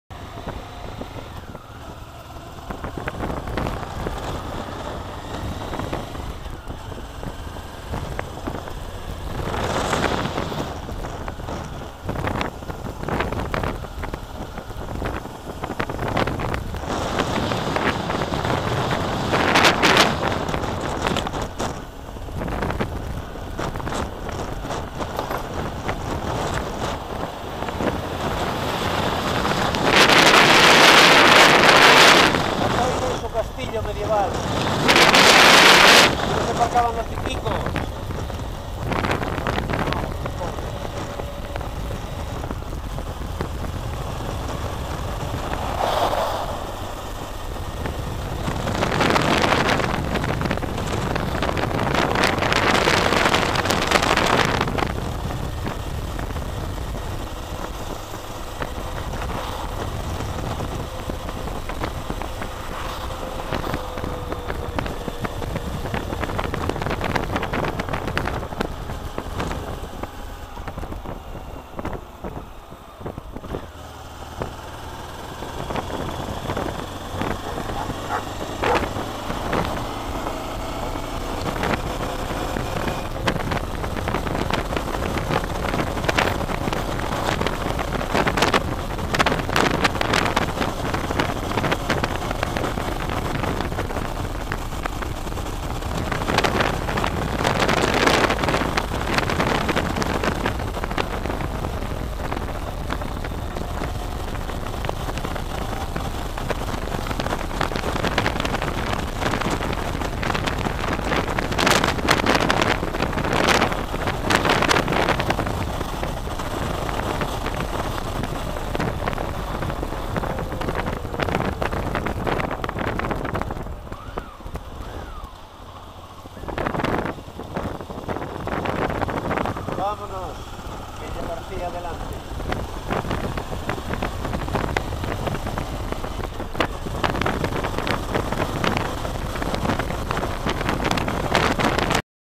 1 Catoira
Llegando en moto a Catoira, sobre el puente de la ría de Arousa